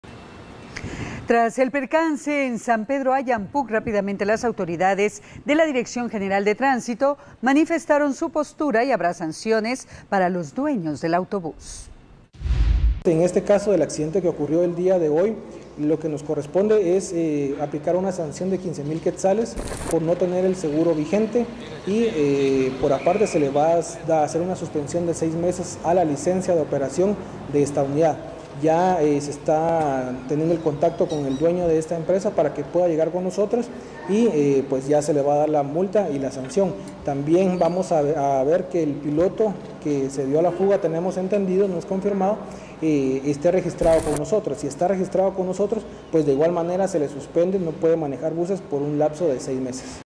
Autobús accidentado en San Pedro Ayampuc será multado con 15 mil quetzales
Tras el percance en San Pedro Ayampuc, rápidamente las autoridades de la Dirección General de Tránsito manifestaron su postura y habrá sanciones para los dueños del autobús. Piloto no tenía seguro vigente.